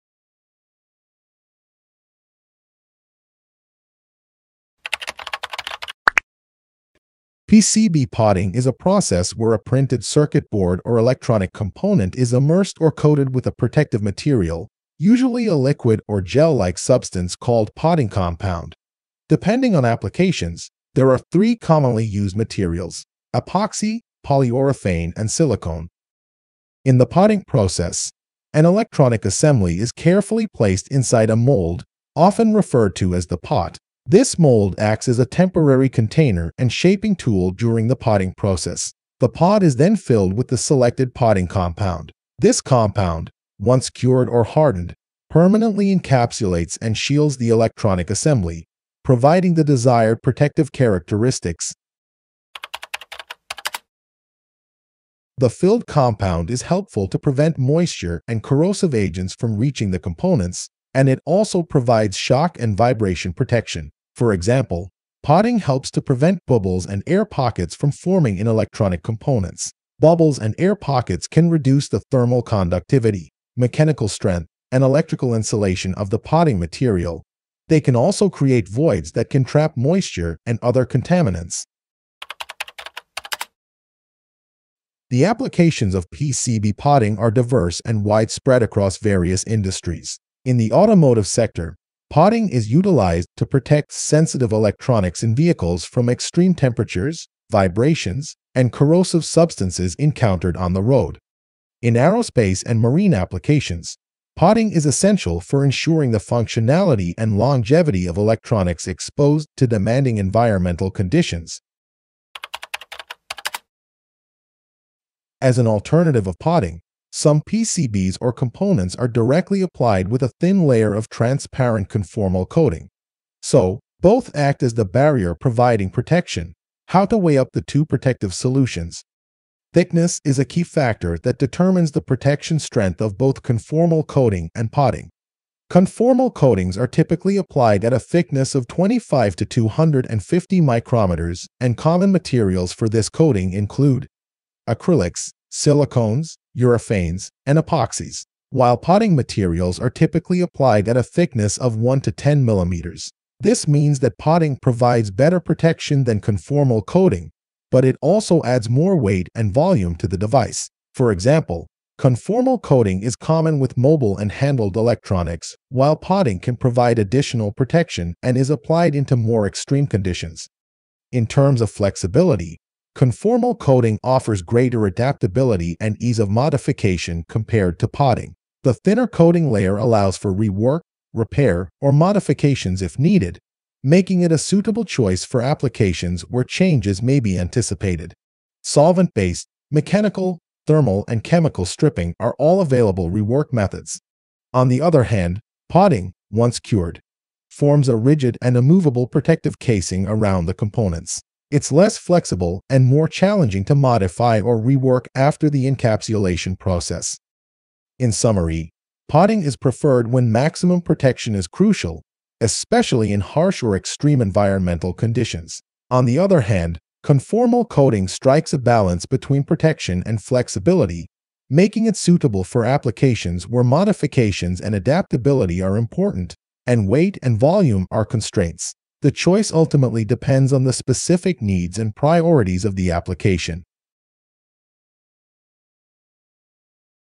PCB Protection: Potting or Conformal Coating? | PCB Knowledge
PCB potting is a process where a printed circuit board or electronic component is immersed or coated with a protective material, usually a liquid or gel-like substance called potting compound.
Together with coating, they are both methods to protect circuit boards. Do you know the difference between the them? Find more information in the video.

Chapters:
00:00 Intro
00:53 Advantages of PCB Potting
01:22 Applications of PCB Potting
01:52 How does potting compare to conformal coating?
02:11 Protection strength
02:52 Flexibility and Rework